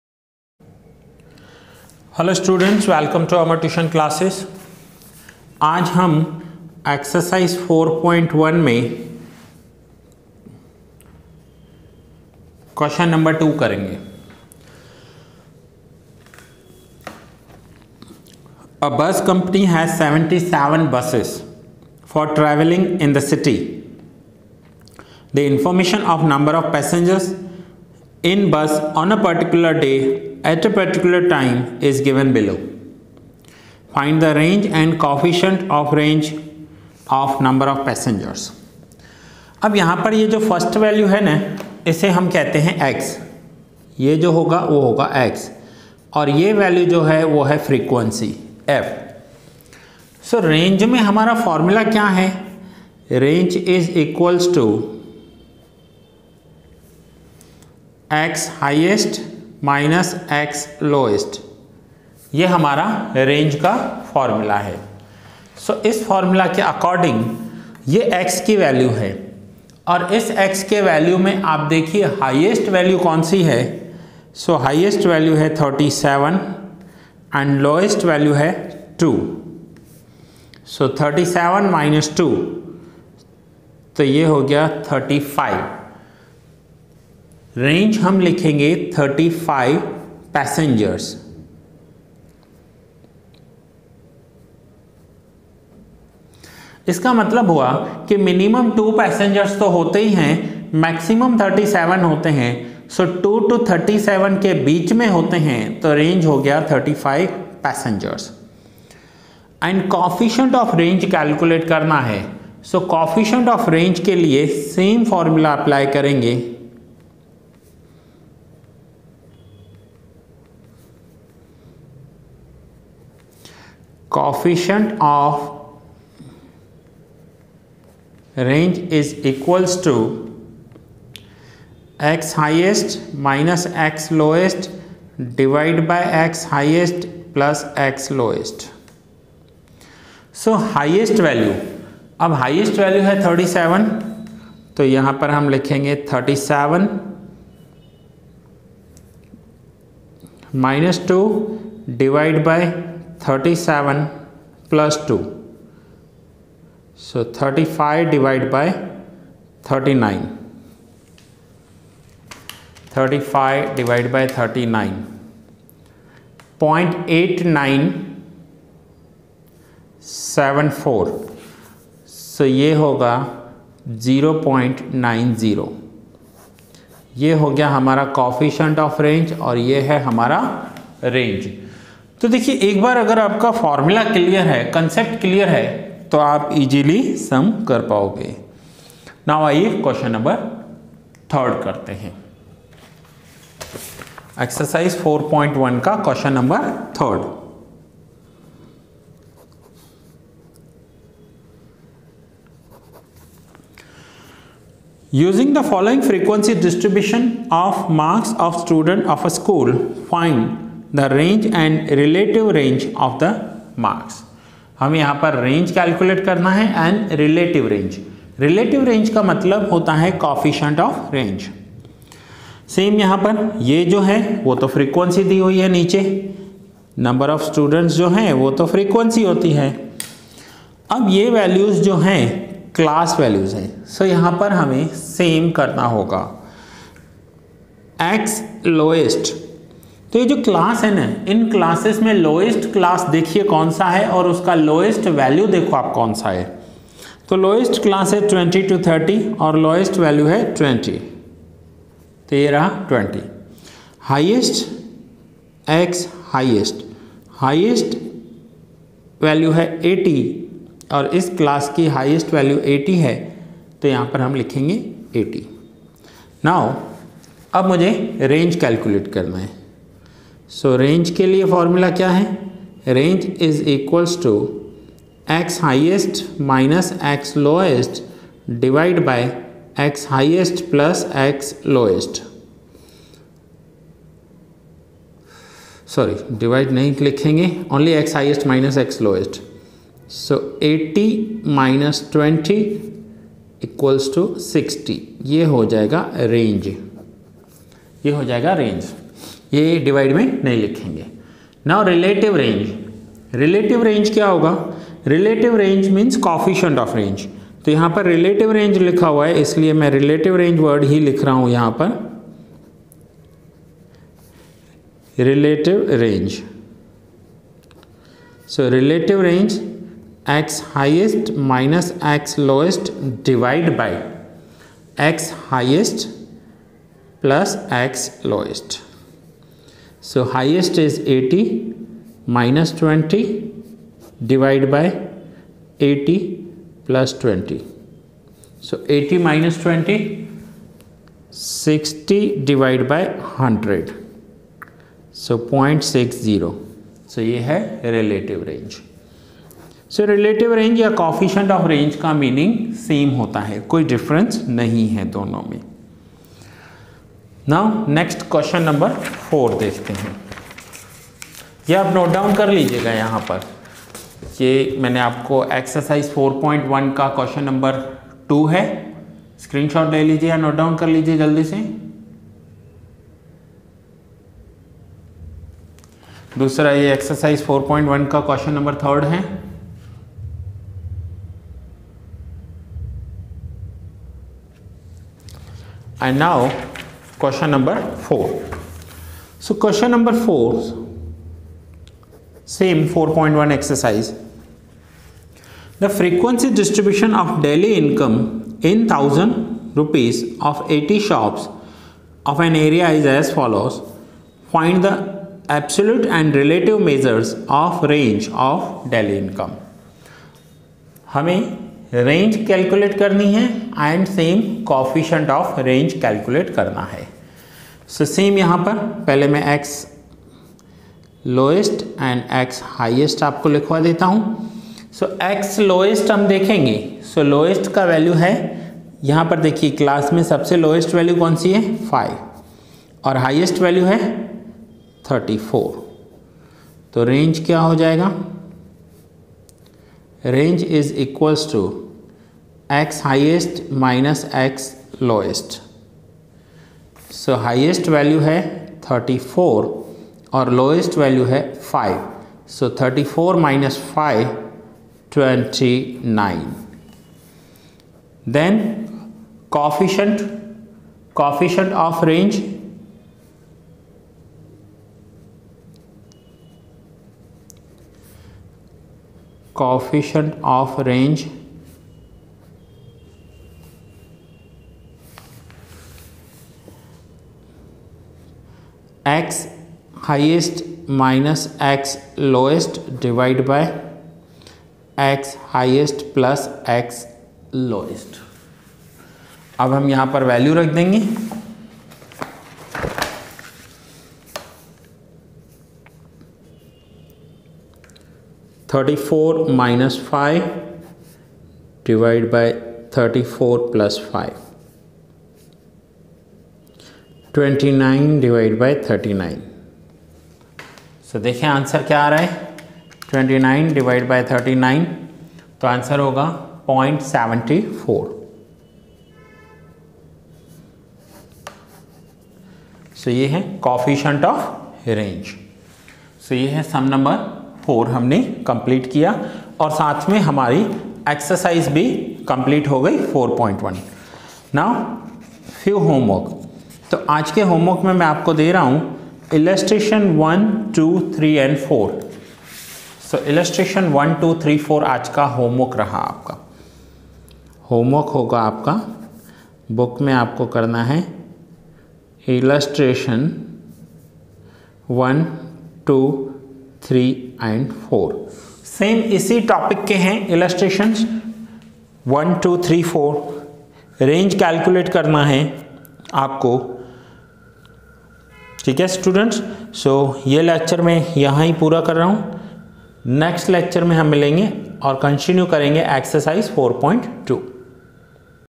Lecture-2, Statistics, Ch-4 of 11th GSEB | Measures of Dispersion in Hindi by Amar Sir
Amar Tuition Classes,
Shop No.11, First Floor,
Auda Market,
Opp. Shikarpuri Sweets,
Sardarnagar,
Ahmedabad.